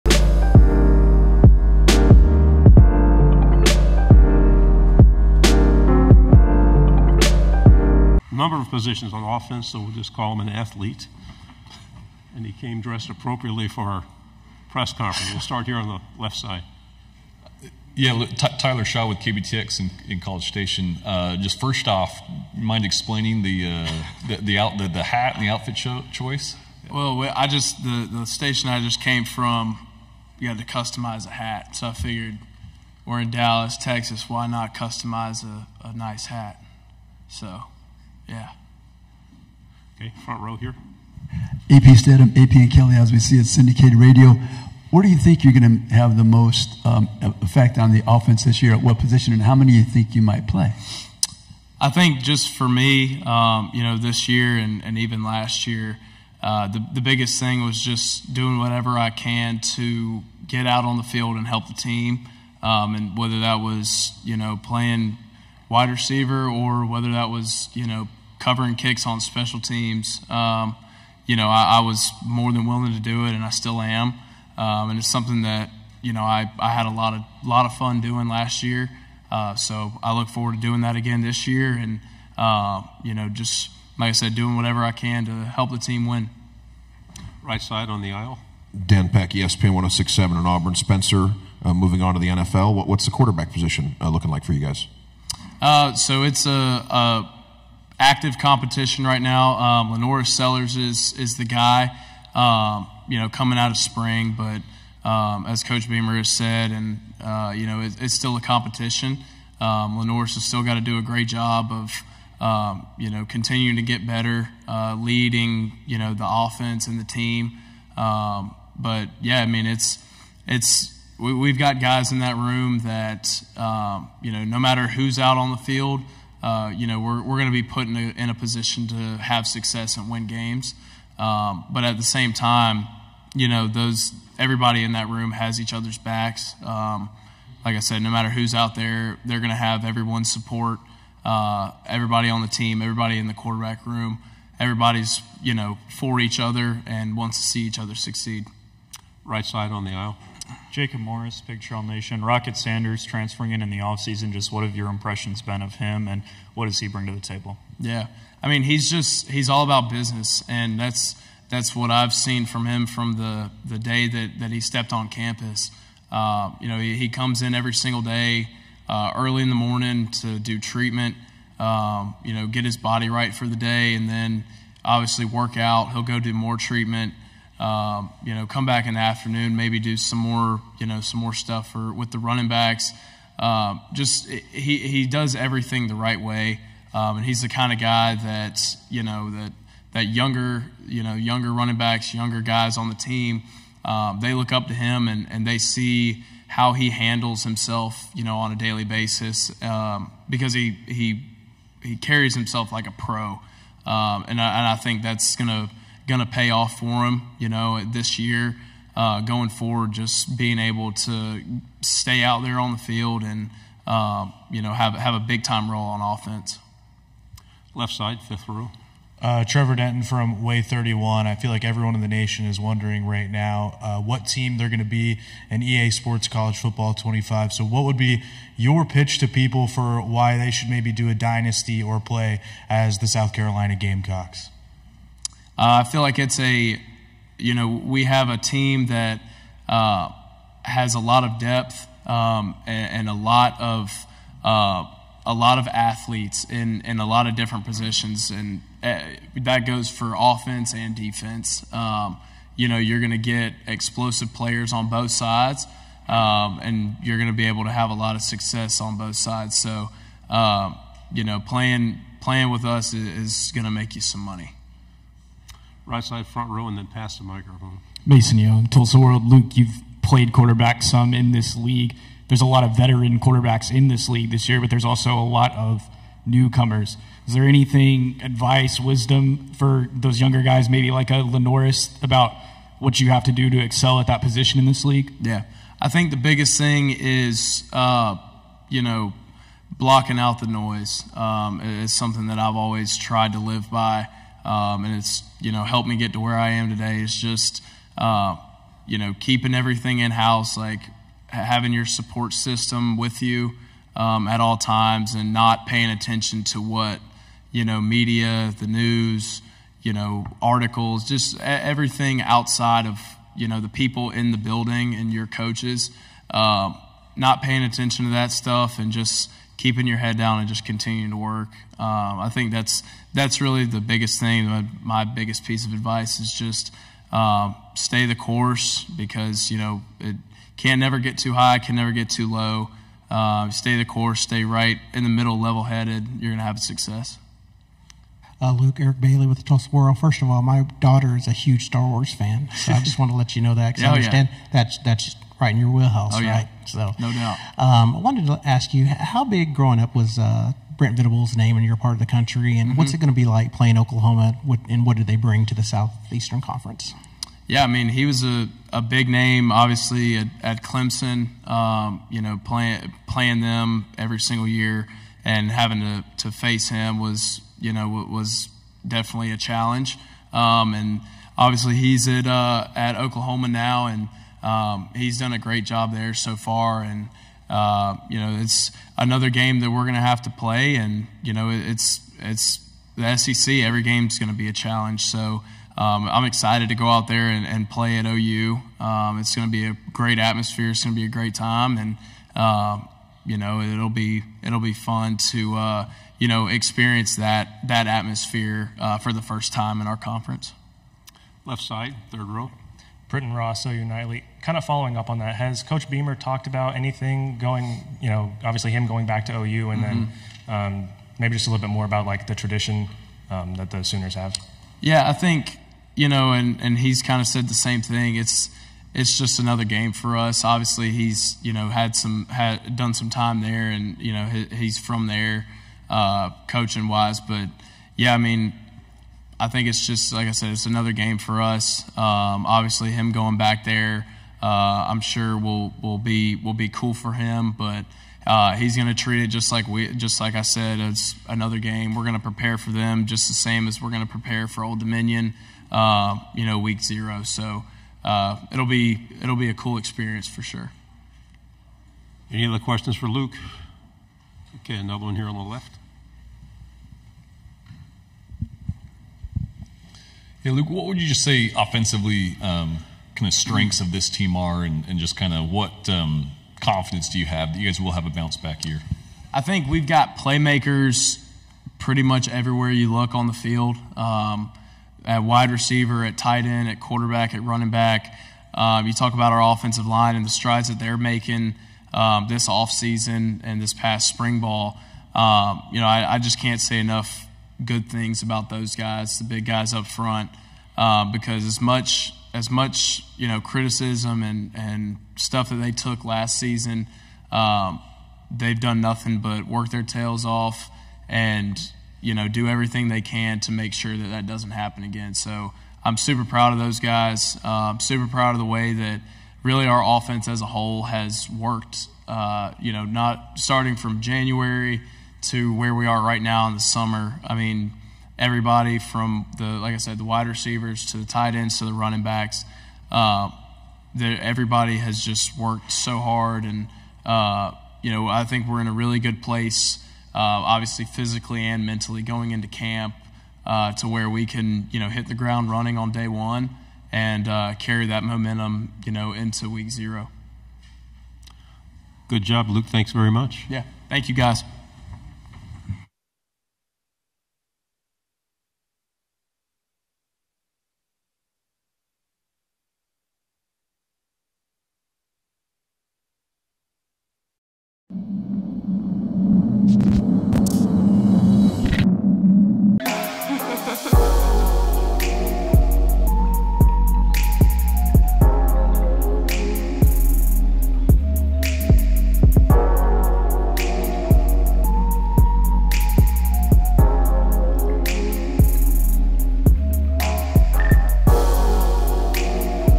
Luke Doty Discusses State of South Carolina Gamecocks Football, QB Situation at SEC Media Day 2024
Watch as Luke Doty Discusses State of South Carolina Gamecocks Football, QB Situation at SEC Media Day 2024